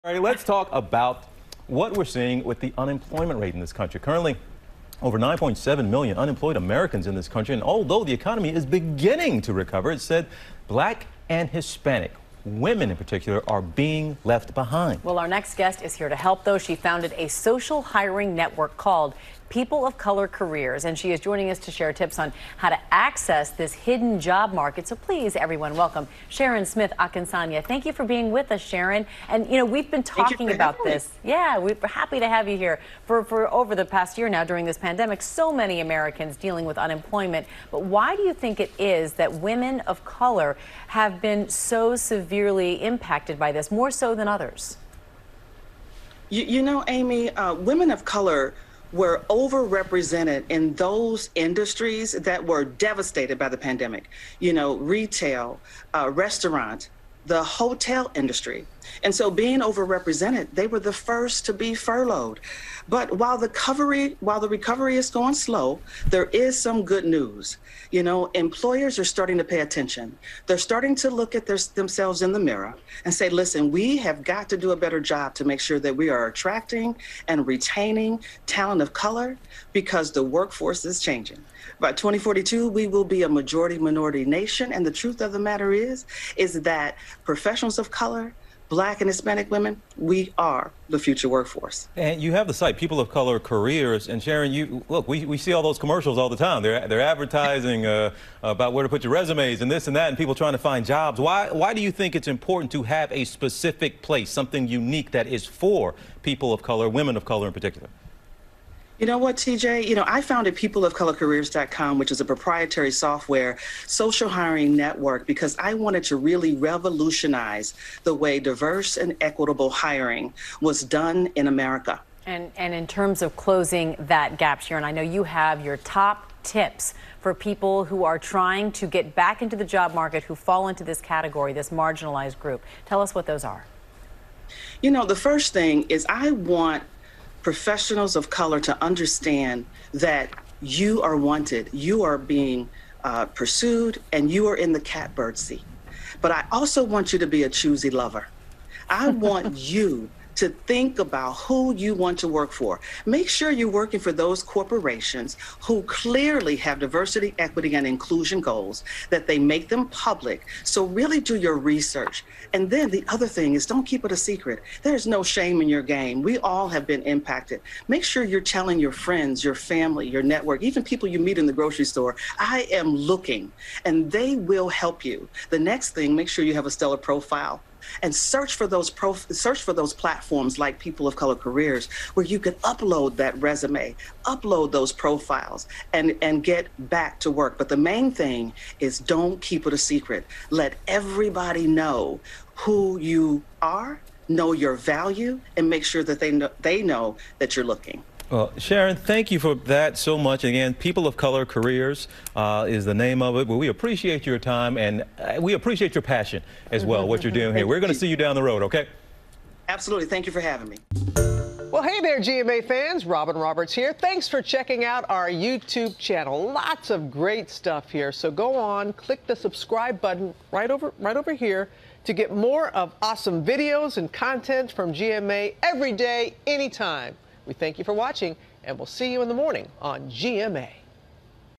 How BIPOC can access the hidden job market
Diversity expert Sharon Smith-Akinsanya helps top employers build more authentic relationships with people of color.